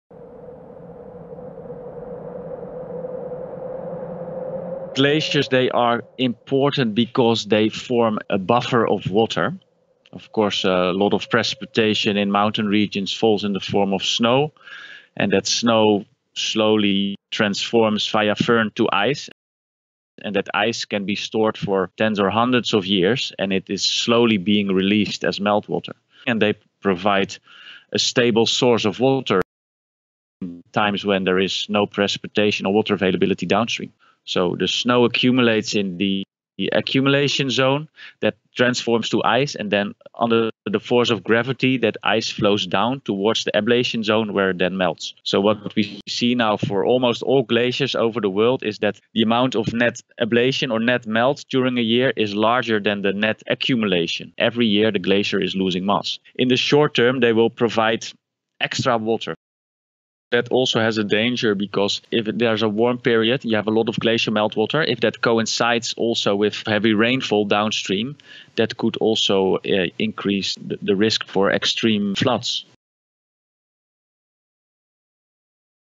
One-minute science: Why do mountain glaciers matter?
Glaciers provide a crucial water supply for people and animals who live nearby but they’re shrinking ➡️